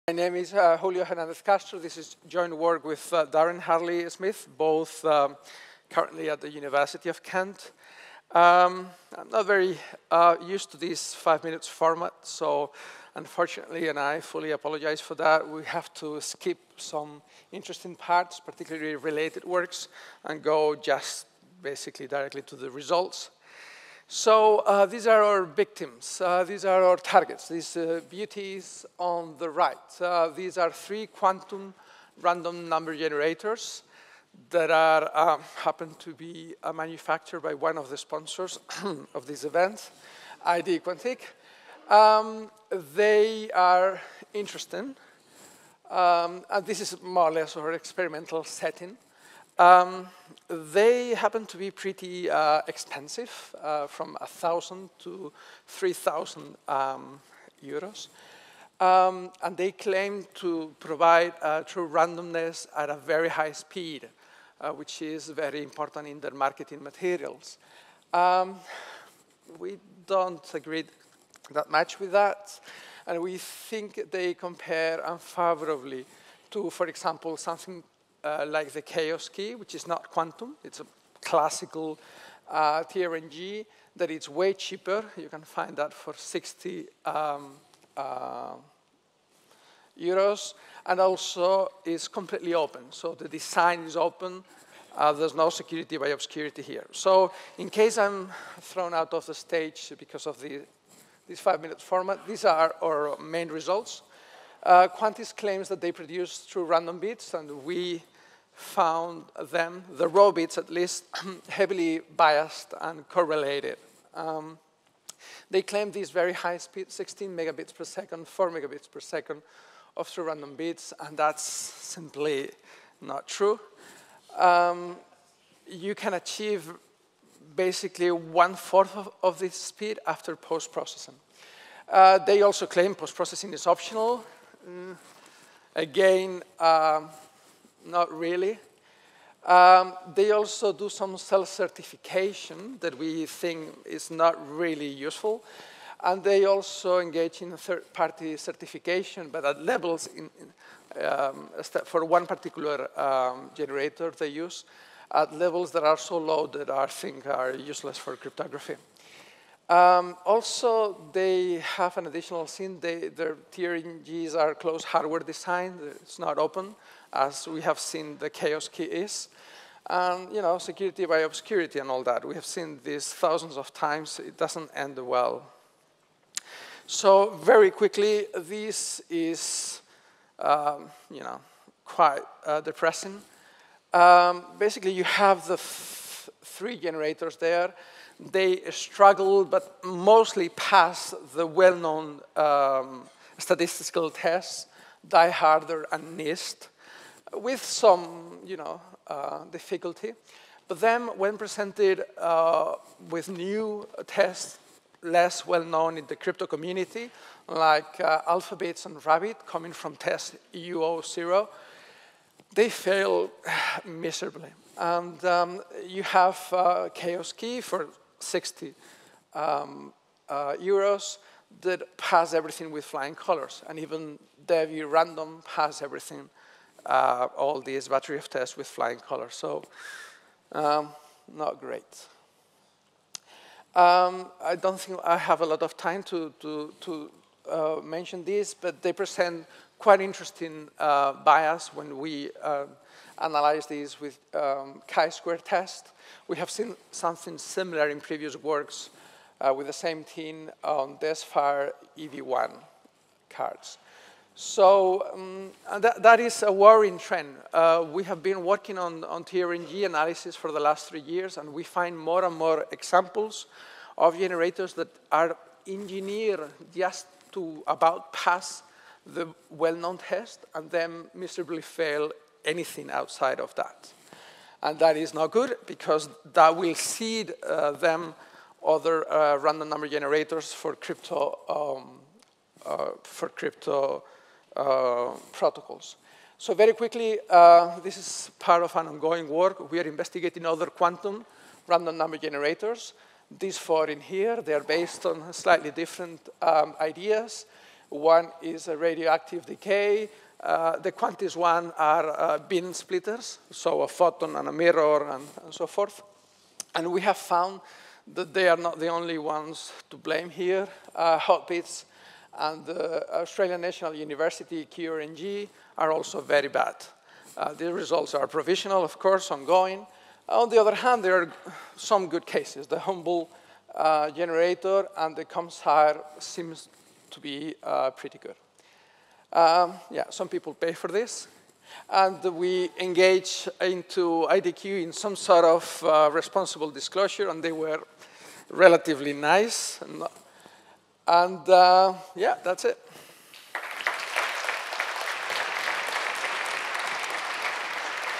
Quam Bene Non Quantum: identifying bias in a commercial quantum random number generator
Technical talks from the Real World Crypto conference series.